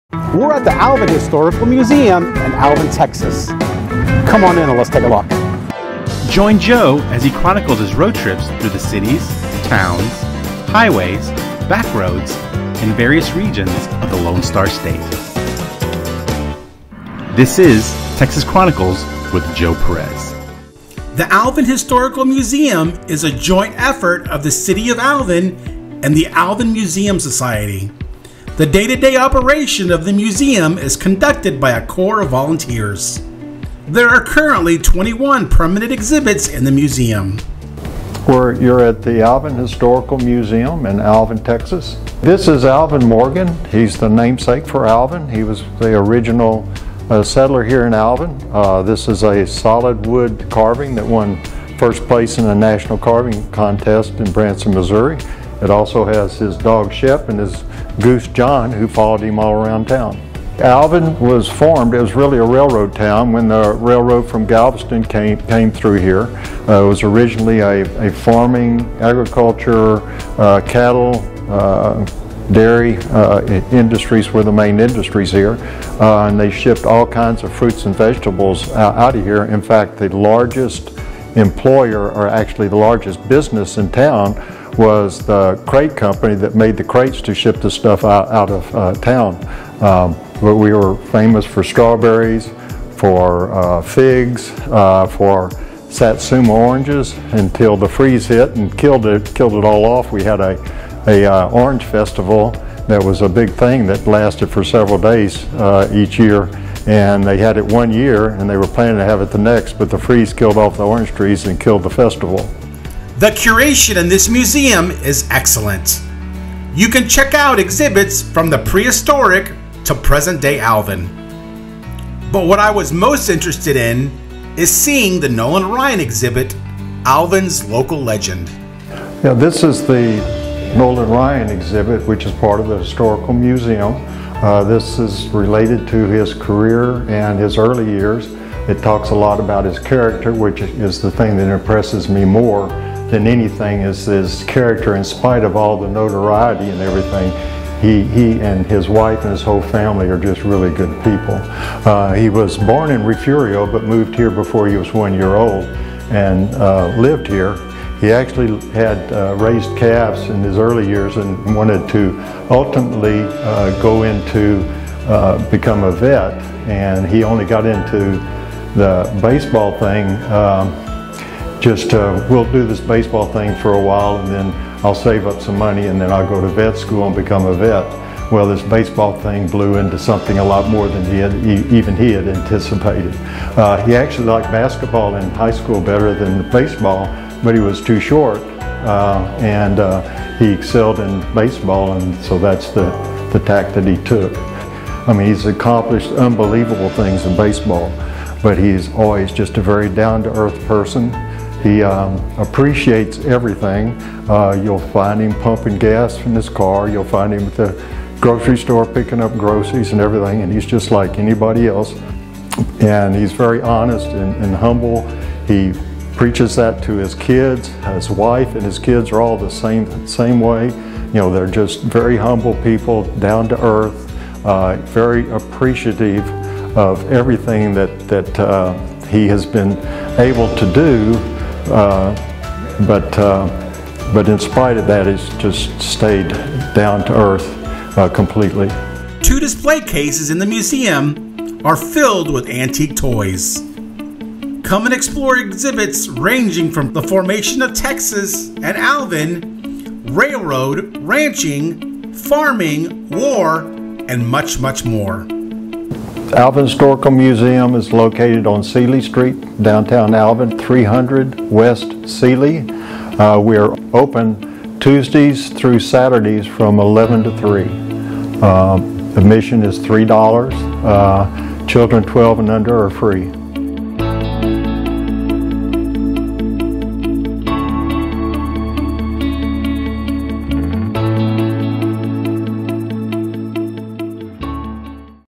Texas Chronicles: Alvin Historical Museum's Nolan Ryan Exhibit
Our next adventure takes us to Alvin, Texas where we’re visiting the Nolan Ryan Exhibit at the Alvin Historical Museum . The Alvin Historical Museum serves as a casual destination for area residents as well as history buffs, tourists, and special exhibit visitors. The Nolan Ryan exhibit contains over 40 feet of graphics and artifacts related to Nolan Ryan.